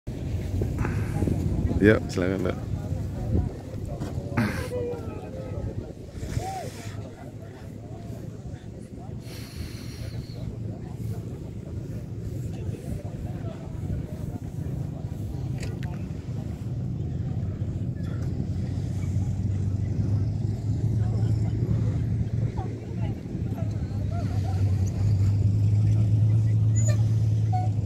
Bromo, Minggu 22 Mei 2022. Bersama rombongan Kepala UPT dan Kepala BPSDM Perhubungan.(1)
Bromo, Minggu 22 Mei 2022. Bersama rombongan Kepala UPT dan Kepala BPSDM Perhubungan  acara Rakornis BPSDM Perhubungan di Poltekbang Surabaya.

Bromo, Sunday 22 May 2022. Together with a group of Heads of UPT and Heads of BPSDM Transportation, BPSDM Transportation Coordination Meeting at Poltekbang Surabaya.